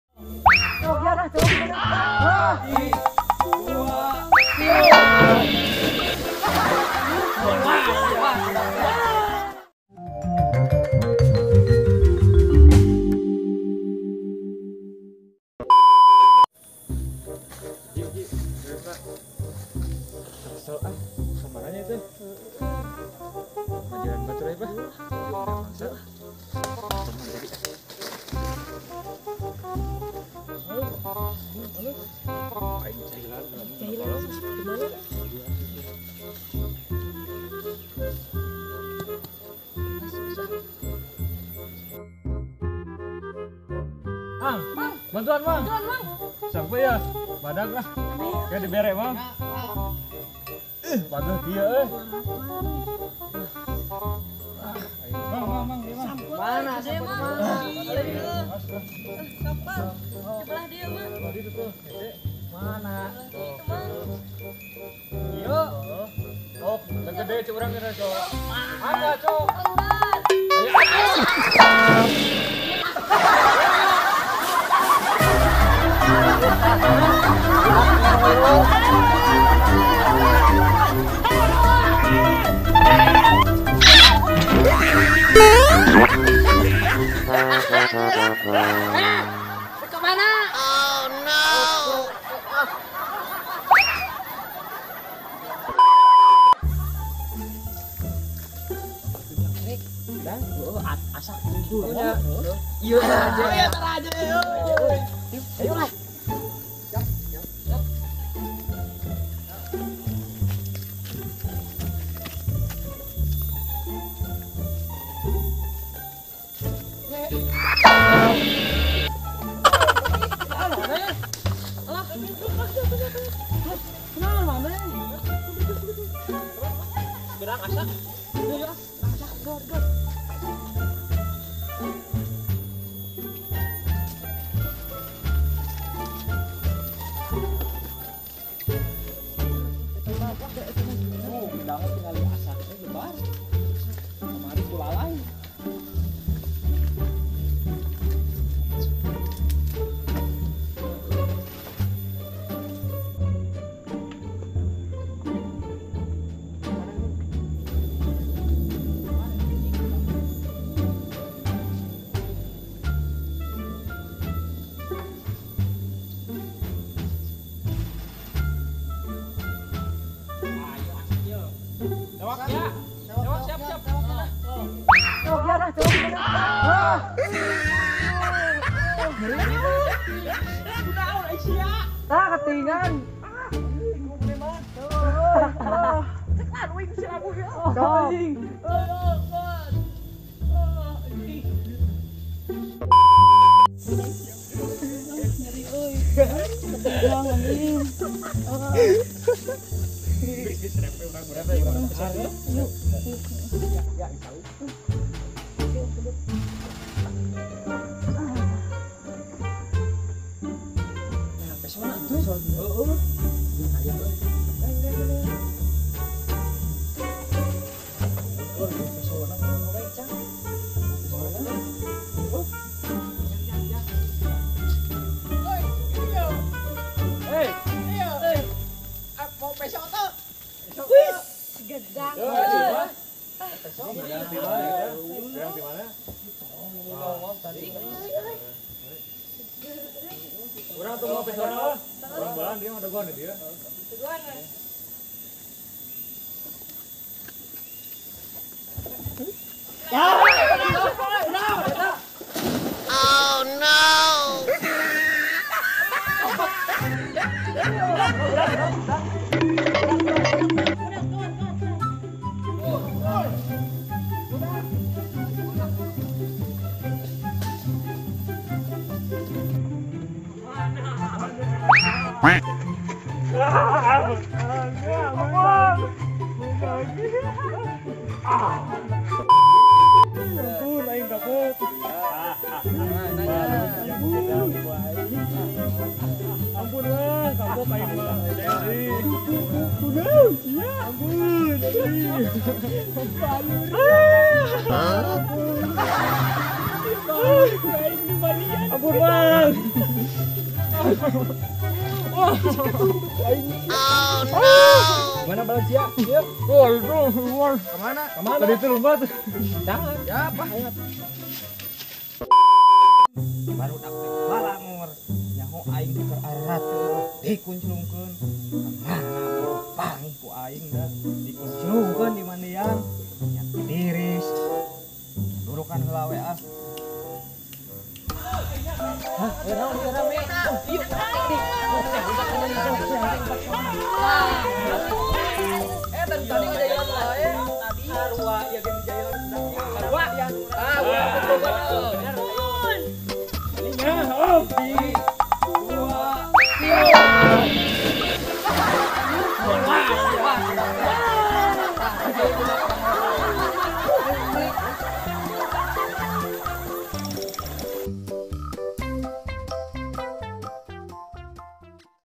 Komedi anak desa salawu E 5 | ANAK NAKAL bikin ulah
ANAK NACKAL | Film pendek lucu | video lucu
Film pendek ini dibuat hanya untuk hiburan semata,
semoga menghibur.